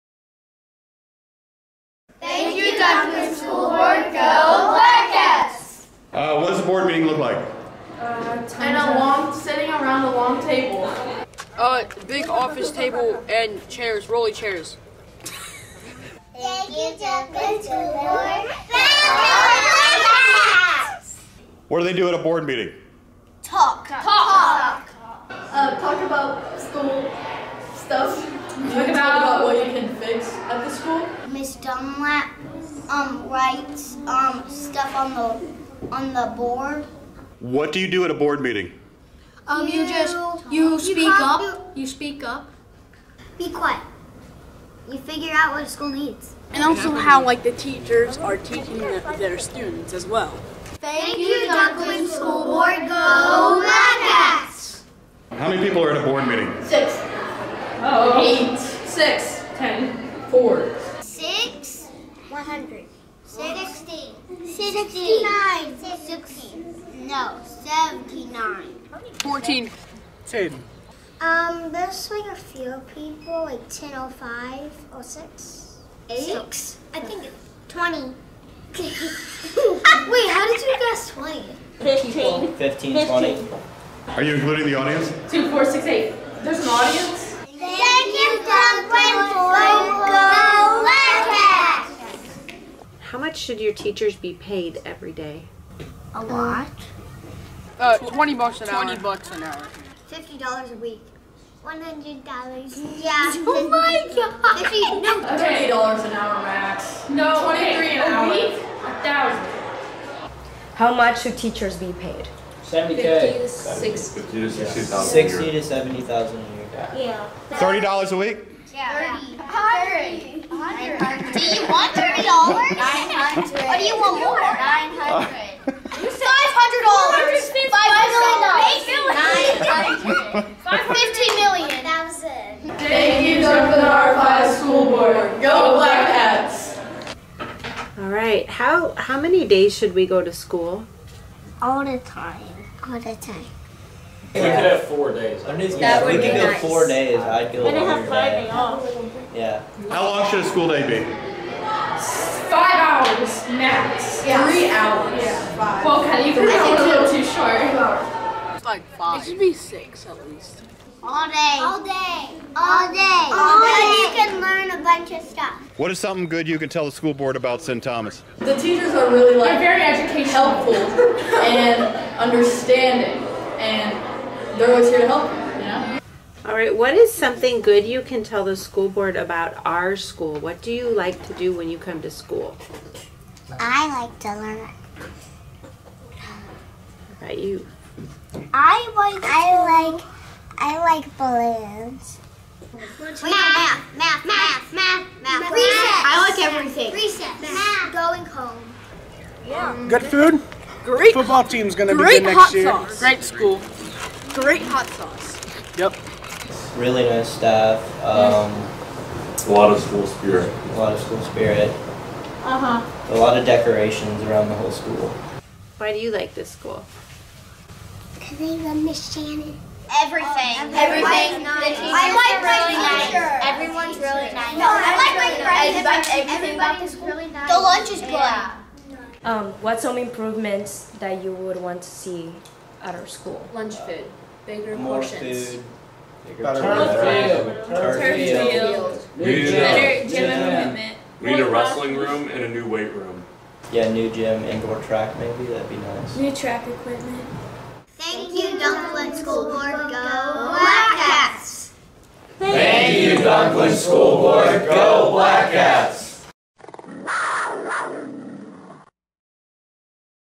Dunklin R-5: School Board Appreciation 2022
Dunklin R-5 students from all our buildings are thanking the school board and answering questions.